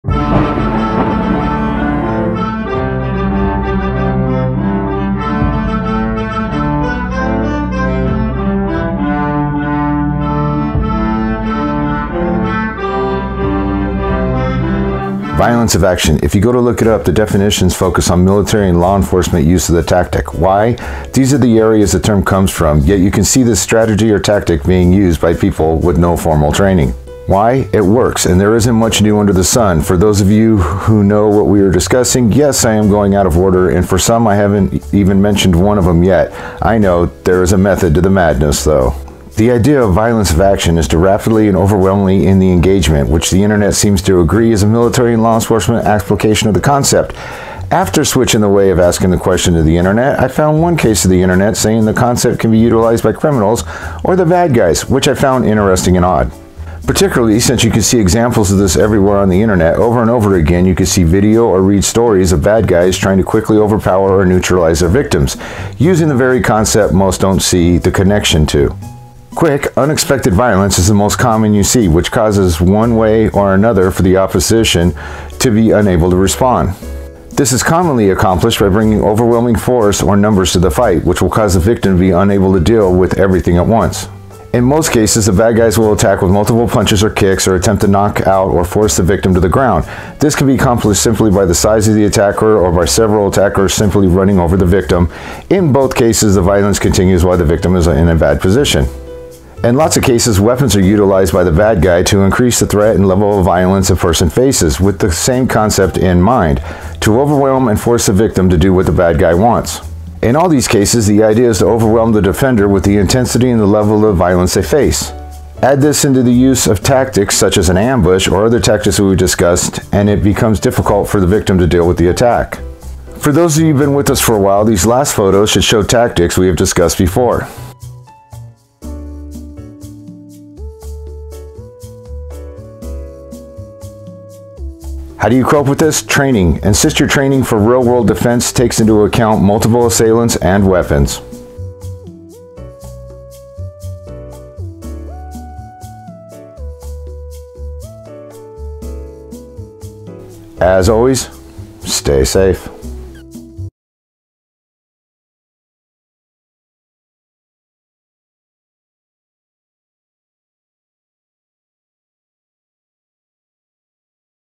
Hard Truth action
Hard Truth about speed of confrontations. What do you need to do to improve your situation. Only you can can fix it!